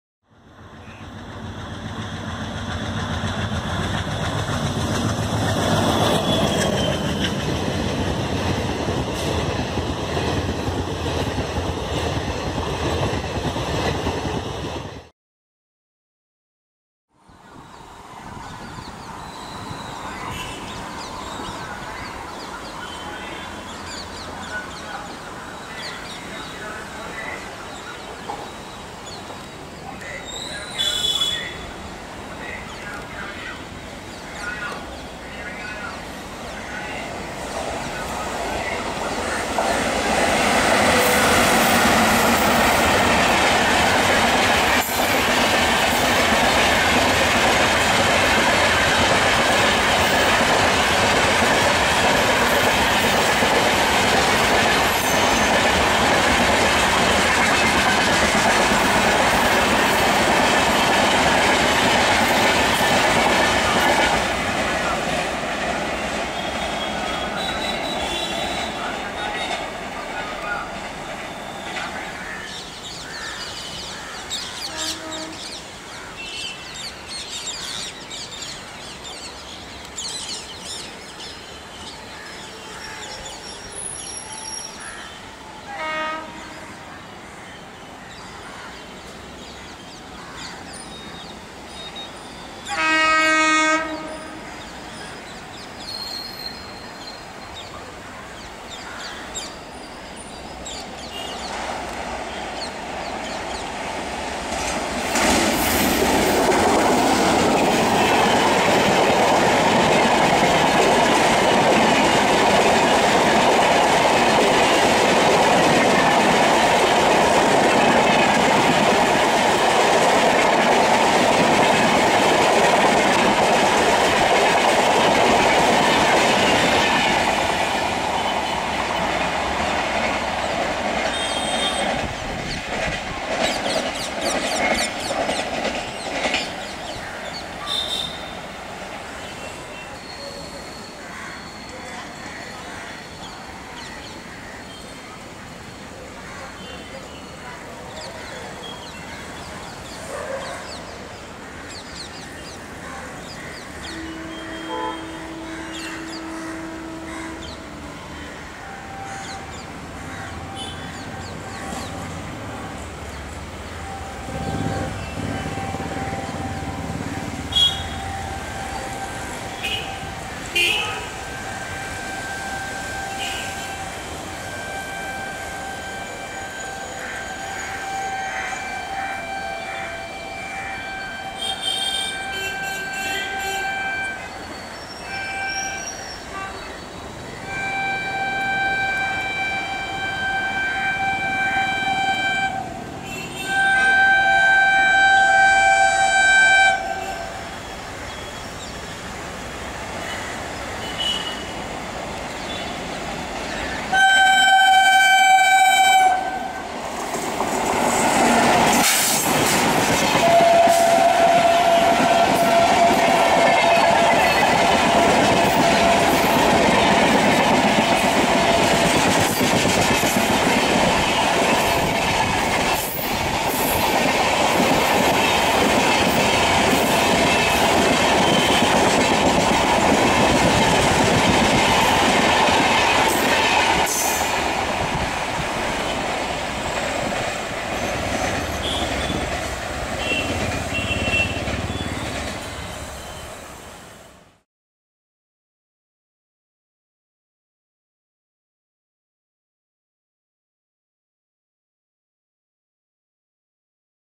Evening LHB Trio with DLW made WAP7s - KANYAKUMARI, CHARMINAR and CHOLAN | COVID-19 Special Trains
3 LHB trains each hauled by DLW-made WAP7 locos from different sheds rattle through Mambalam in fading light -

1) 02633 / Chennai - Kanyakumari Special
2) 02759 / Tambaram - Hyderabad Charminar Special
3) 06796 / Trichy - Chennai Cholan Special

Happy Virtual Railfanning :)

भारतीय रेल